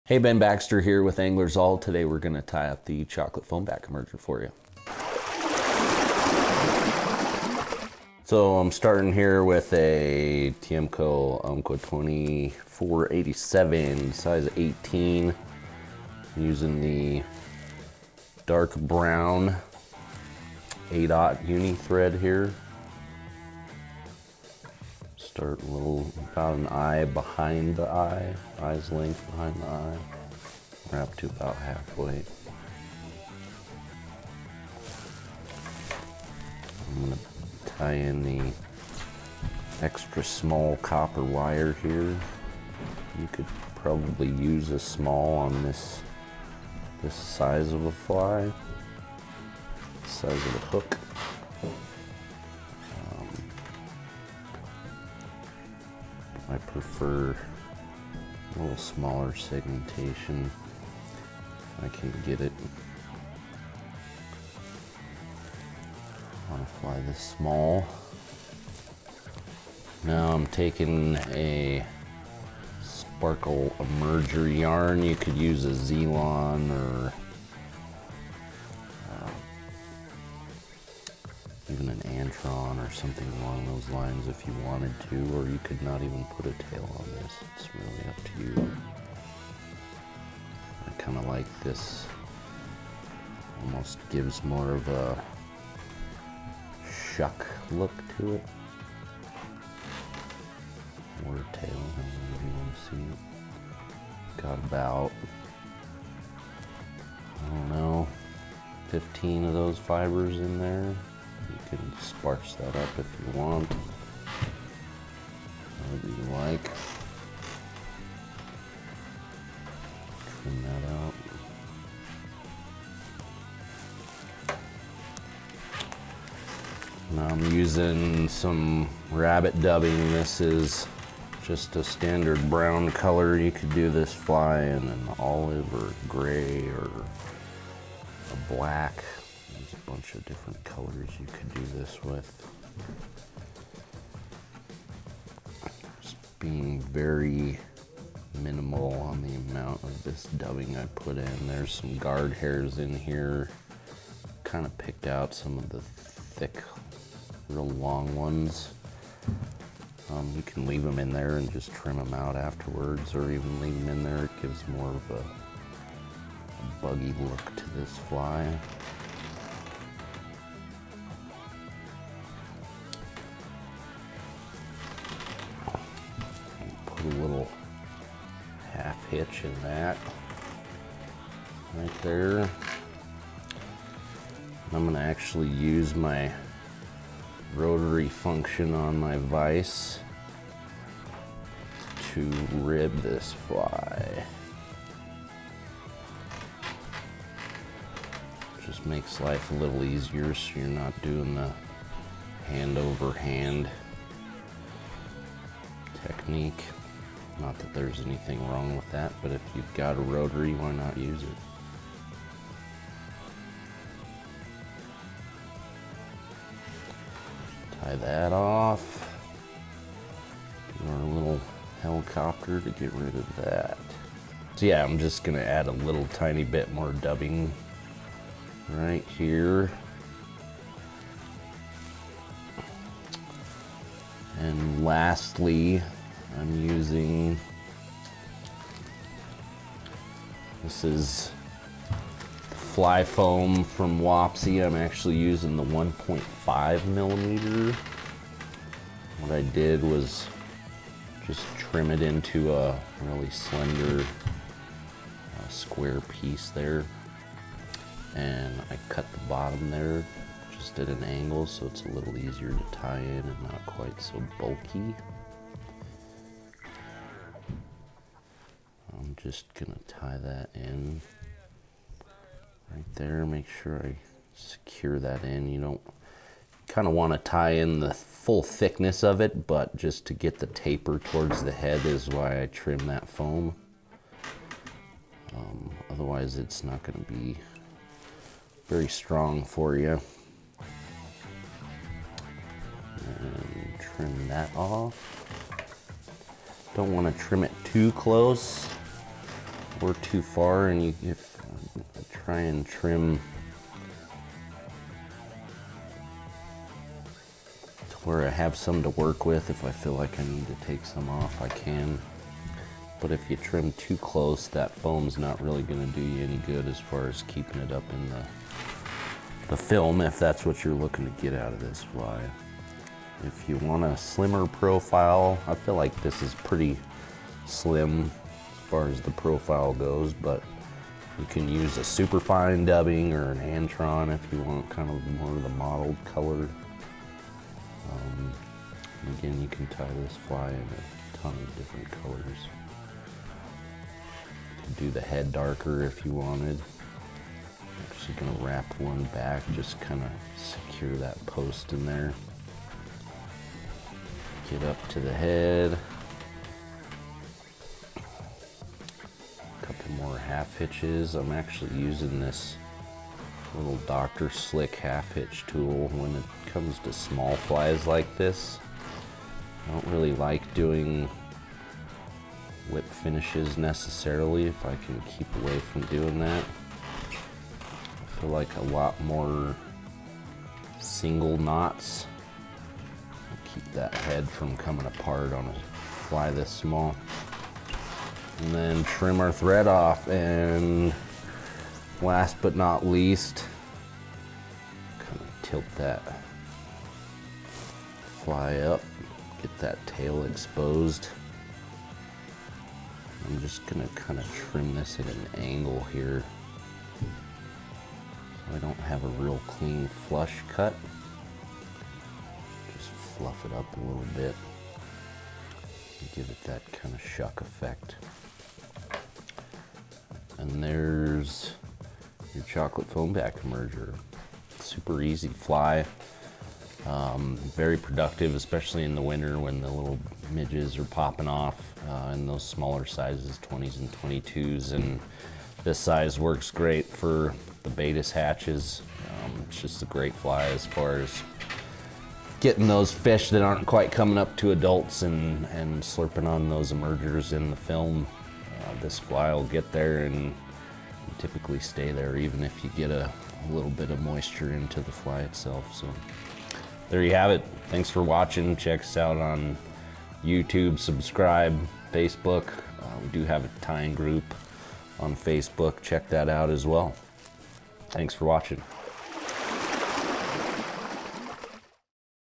Chocolate Foam Back Emerger | FLY TYING TUTORIAL
In this video, resident fly tying expert and Umpqua Feather Merchants Signature Tyer Ben Baxter gives us a step-by-step tutorial on how to tie the Chocolate Foam Back Emerger. This fly is deadly all across the country, but is especially deadly in western tailwater fisheries. If you're fishing the South Platte River during the winter months, the Chocolate Foam Back Emerger is a must-have fly.

MATERIALS LIST: Chocolate Foam Back Emerger

HOOK: Tiemco 2487 (#18 - #22)

THREAD: 8/0 UNI-Thread (Dark Brown)

BODY/THORAX: Wapsi Rabbit Dubbing (Brown)

POST: Wapsi Fly Foam (1/16" [  1.5mm] | White)

RIB: Ultra Wire (XS | Copper)

SHUCK/TAIL: Sparkle Emerger Yarn (Brown)

CHAPTERS / TIME STAMPS: Tying the Chocolate Foam Back Emerger

00:00 - Intro
00:23 - Starting the thread
00:50 - Anchoring the wire
01:10 - Tying in the shuck/tail with Sparkle Emerger Yarn
02:24 - Building the body with Rabbit Dubbing
02:56 - Adding a half hitch for extra security
03:05 - Wrapping the wire
03:44 - Building the thorax with Rabbit Dubbing
03:56 - Tying in the post with white Wapsi Fly Foam
04:10 - Ben's trick for trimming the foam to make it easier to work with
04:51 - Trimming the foam post
05:36 - Building up the head with Rabbit Dubbing
06:00 - Adding in several half hitches instead of using a whip finish
06:31 - Trimming the shuck/tail